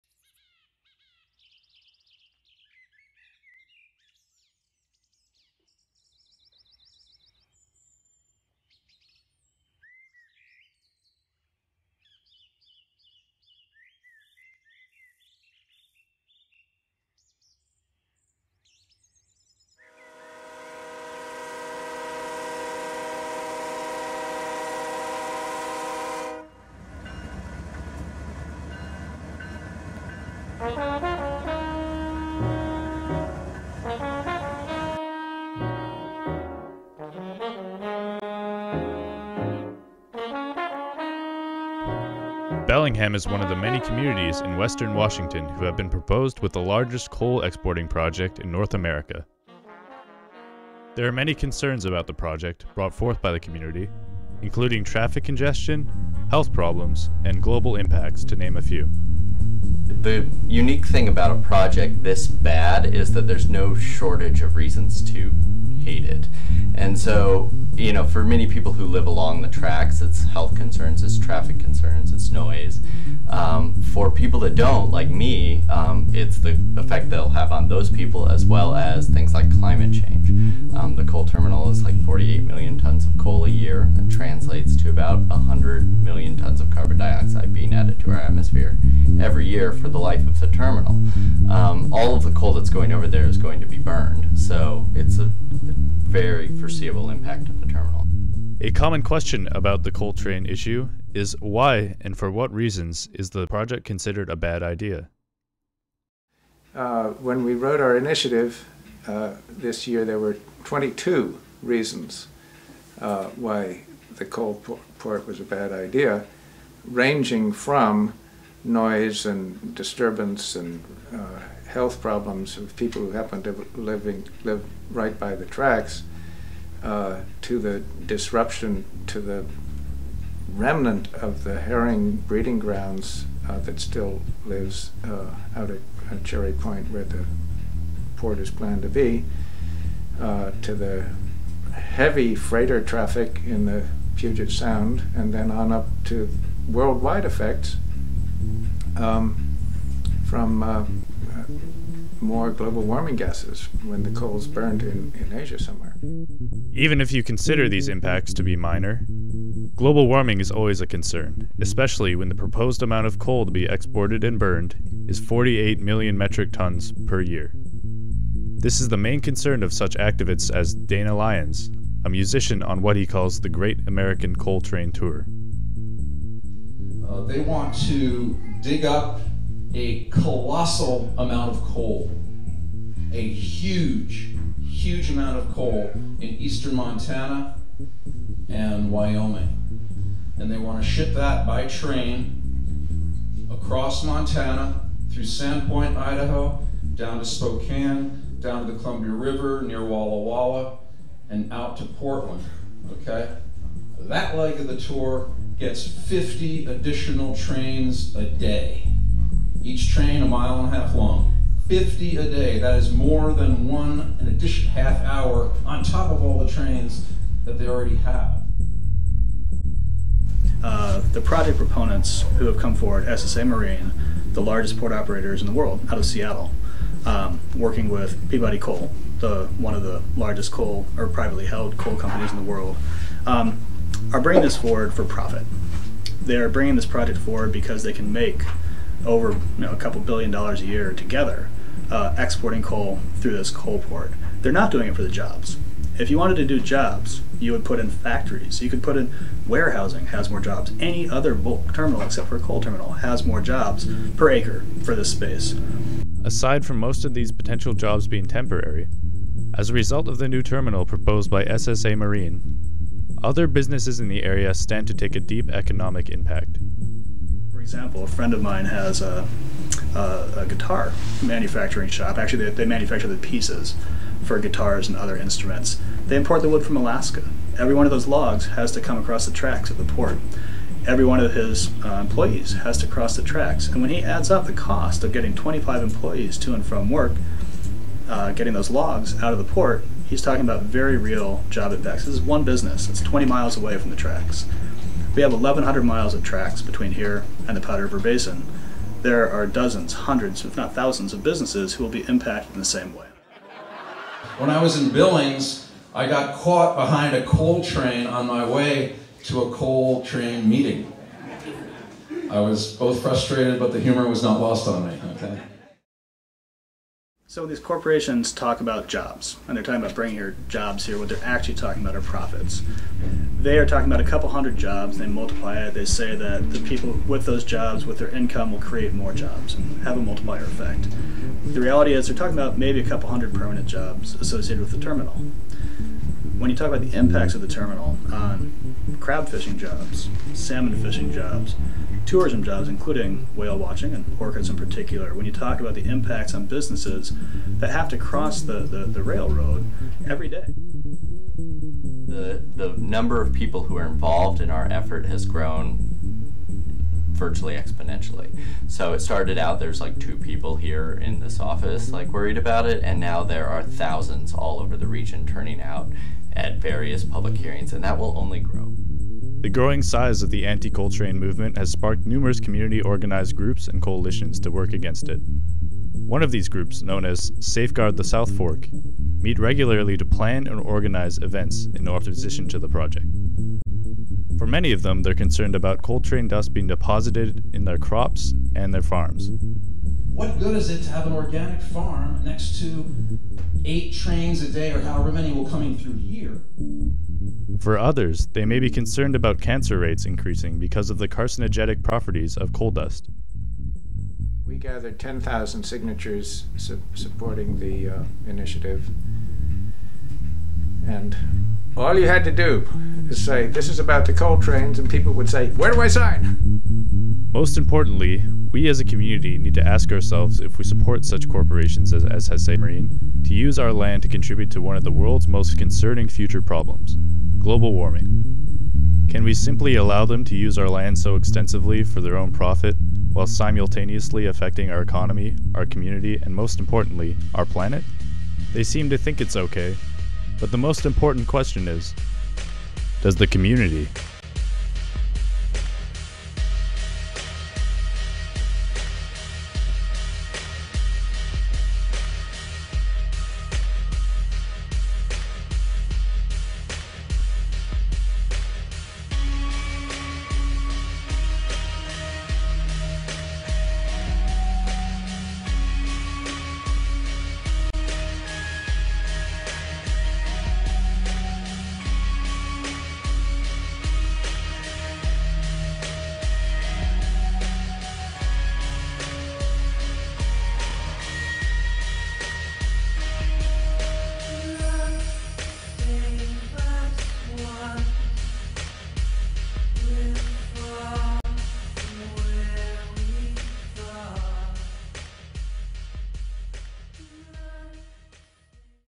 The Great COALition - Culminating Project Documentary
A small documentary outlining the key issues and concerns involved with Whatcom County's local coal train debate.